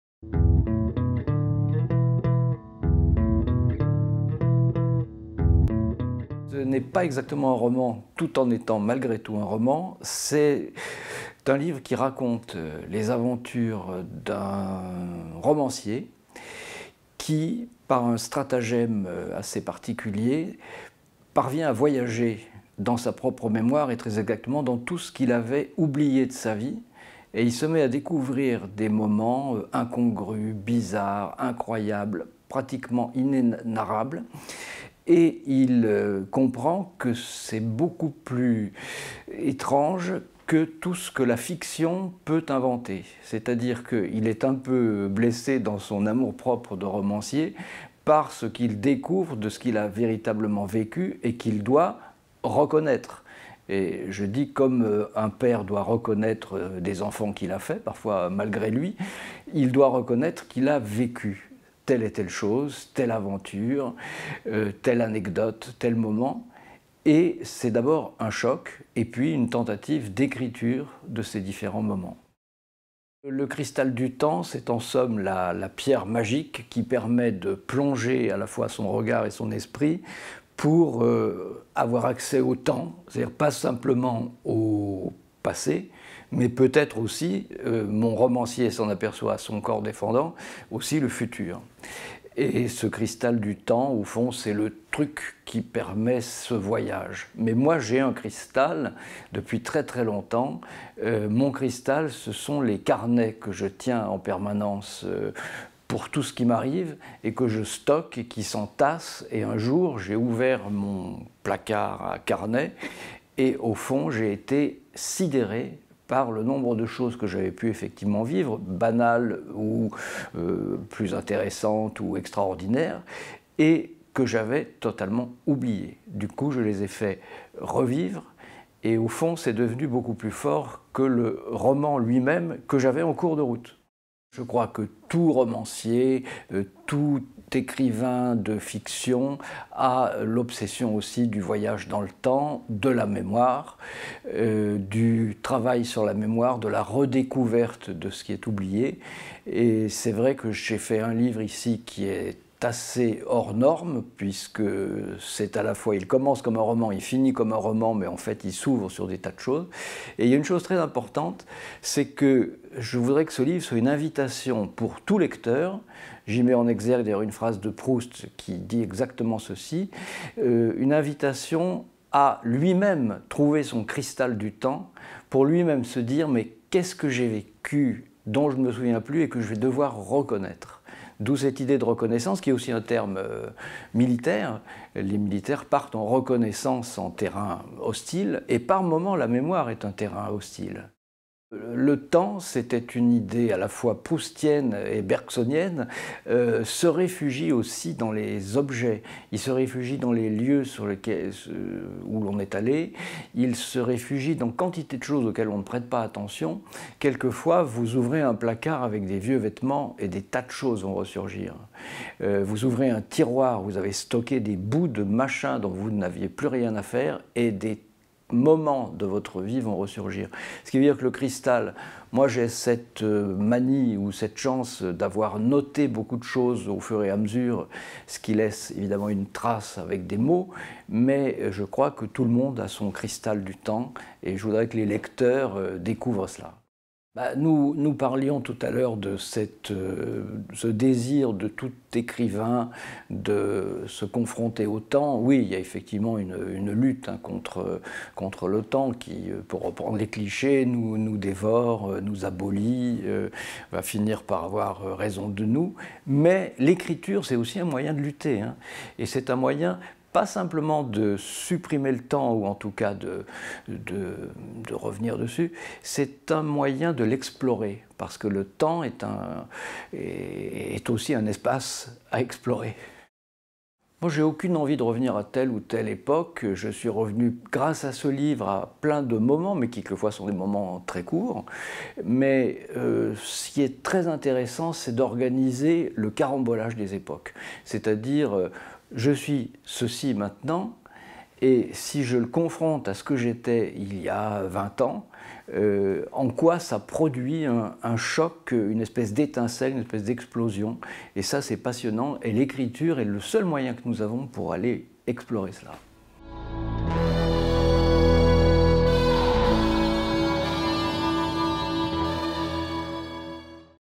Pierre Péju - Reconnaissance
Pierre Péju vous présente son ouvrage "Reconnaissance" aux éditions Gallimard. Rentrée littéraire janvier 2017.